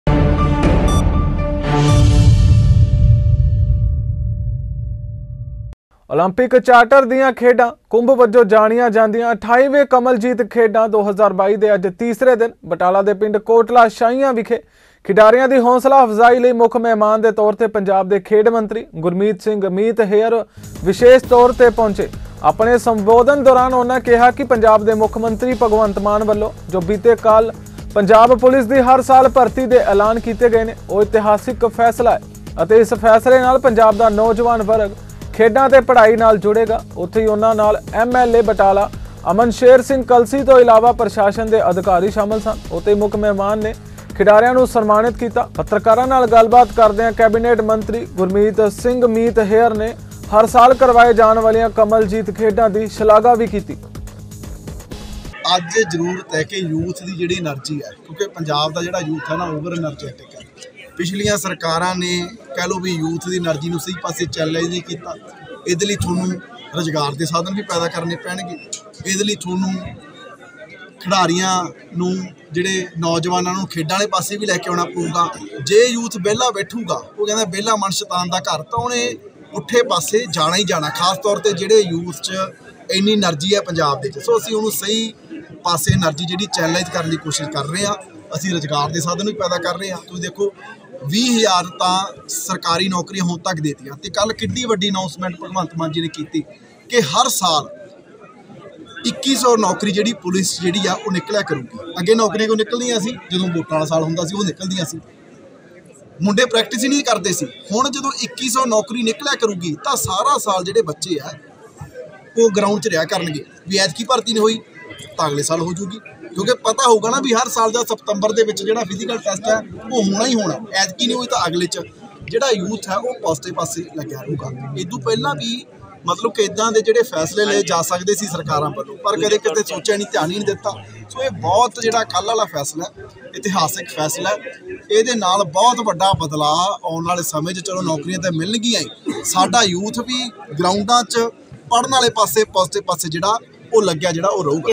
28ਵੇਂ ਕਮਲਜੀਤ ਖੇਡਾਂ-2022 ਦੇ ਅੱਜ ਤੀਸਰੇ ਦਿਨ ਖੇਡ ਮੰਤਰੀ ਗੁਰਮੀਤ ਸਿੰਘ ਮੀਤ ਹੇਅਰ ਵਿਸ਼ੇਸ ਤੌਰ ਤੇ ਬਟਾਲਾ ਪਹੁਚੇ
28ਵੇਂ ਕਮਲਜੀਤ ਖੇਡਾਂ-2022 ਦੇ ਅੱਜ ਤੀਸਰੇ ਦਿਨ ਖੇਡ ਮੰਤਰੀ ਗੁਰਮੀਤ ਸਿੰਘ ਮੀਤ ਹੇਅਰ ਵਿਸ਼ੇਸ ਤੌਰ ਤੇ ਬਟਾਲਾ ਪਹੁਚੇ